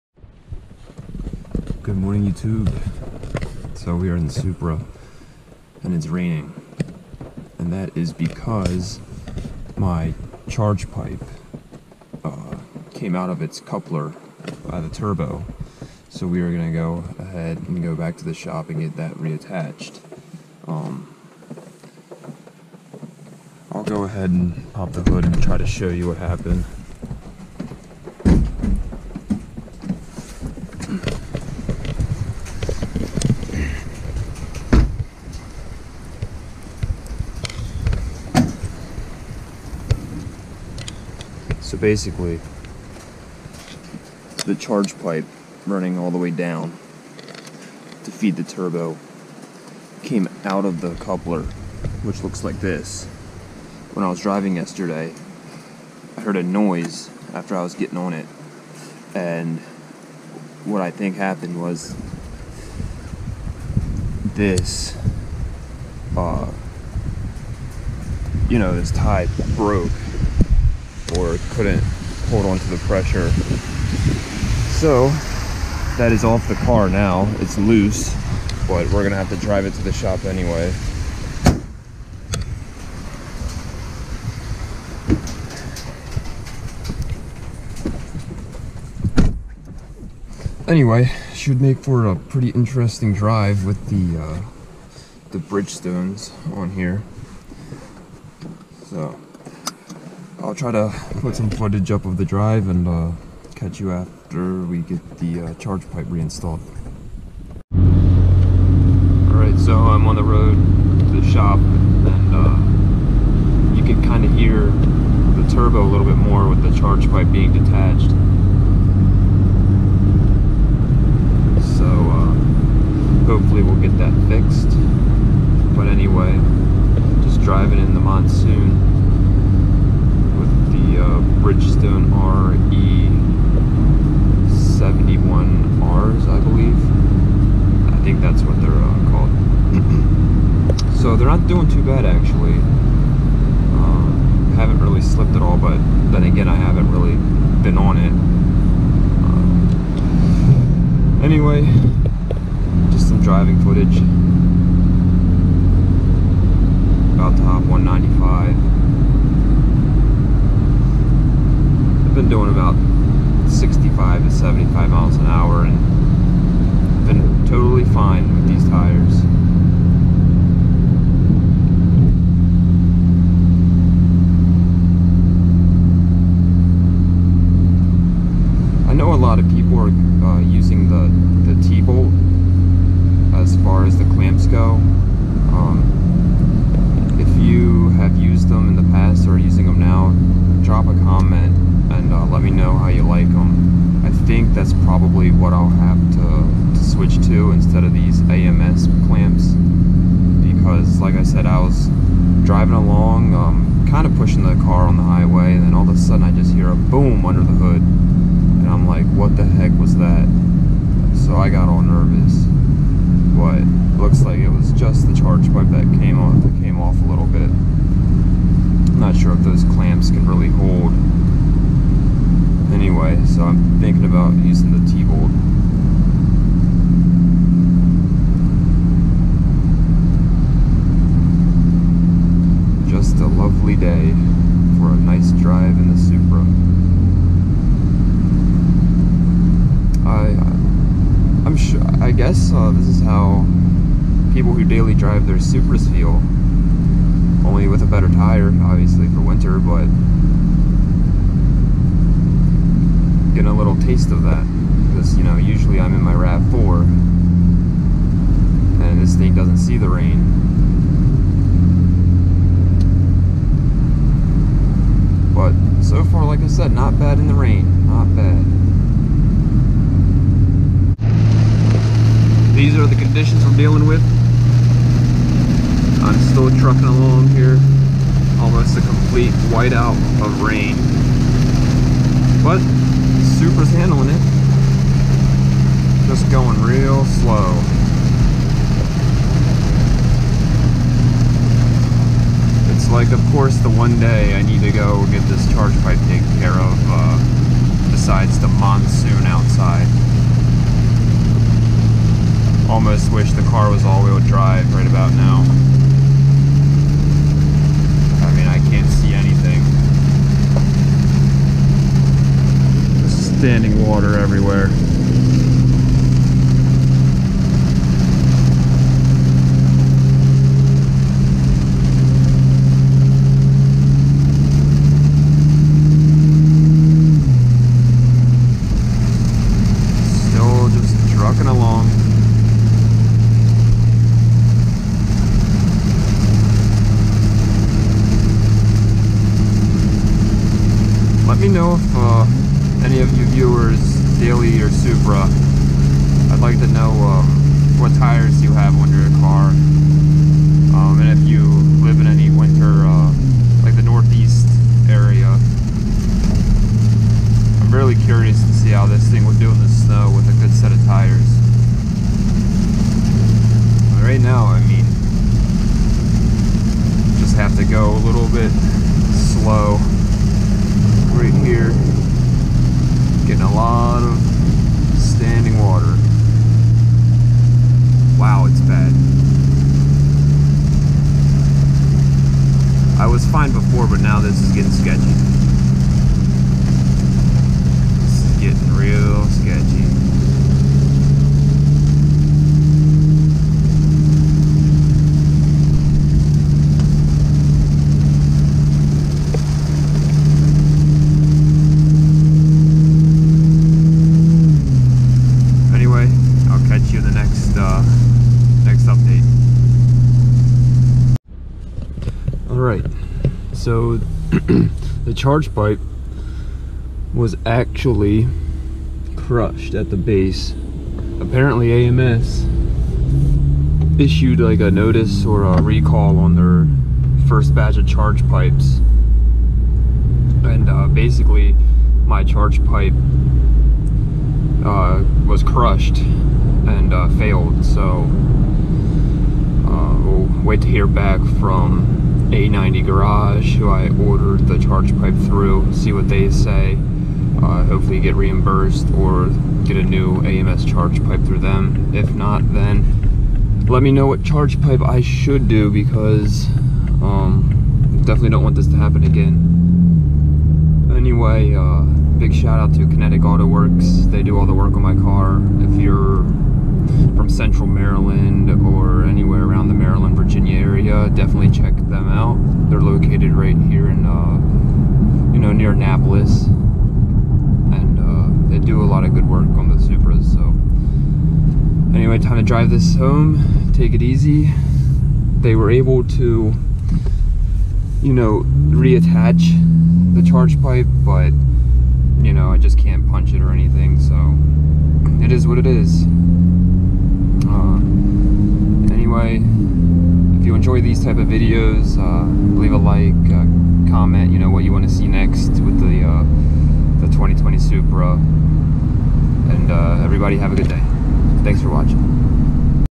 AMS Charge Pipe FAILURE on my Supra!
So yesterday I did a 5th gear pull and I hear a loud "boom" under the hood. What happened was the base of the charge pipe got crunched due to the boost of the turbo. I was unaware that AMS had just recently put out a notice about the first batch of charge pipes were prone to failure. Should I get a new updated AMS charge pipe or go with a different brand?

Shoutout to Kinetic Autoworks for taking care of me.